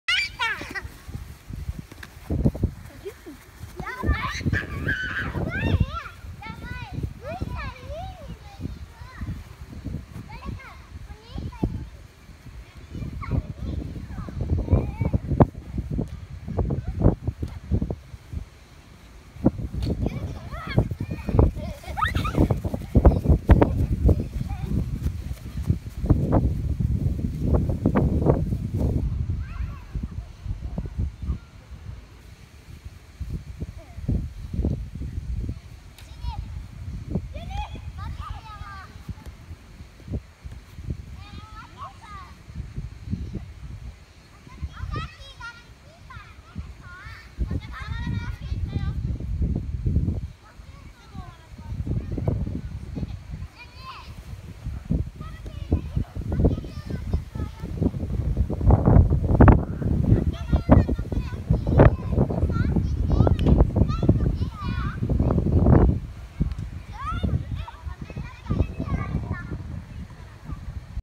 沖縄 サッカースクール 保育園巡回指導 ディヴェルチール プーマ
２０１６年９月２７日の保育園巡回指導はうるま市にあるあかるい子保育園に行ってきました！